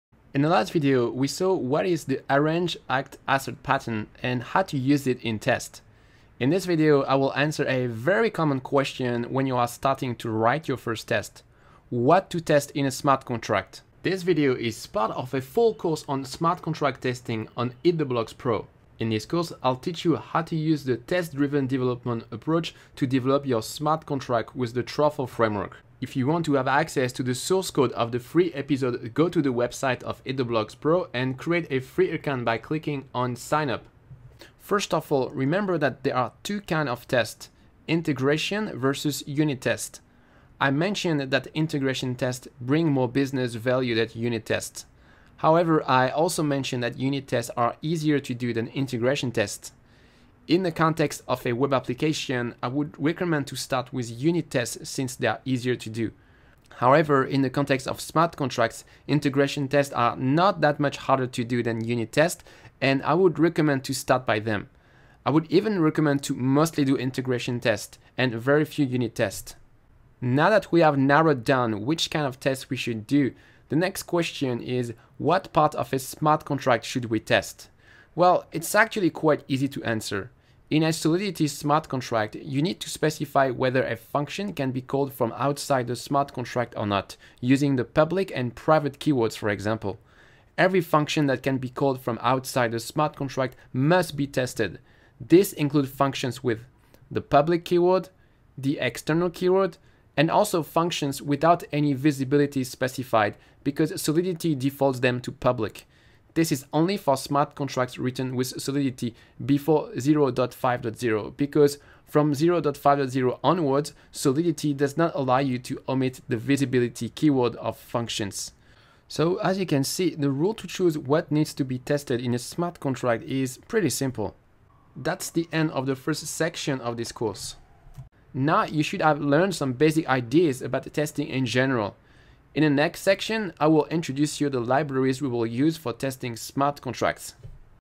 What to test in a Solidity smart contract? | Smart contract testing course
Once you have decided to test your smart contract, one of the first question that comes to mind is:

"WHAT THE HELL SHOULD I TEST???"

That's a very important question! If you can't even answer this question, you risk focusing on the wrong things.

In this video I will explain you what to test in your Solidity smart contracts.

This is part of a full course on Solidity smart contract testing, where I teach how to develop smart contract with the TDD (test-driven-development approach), using the Truffle framework.

(click on signup)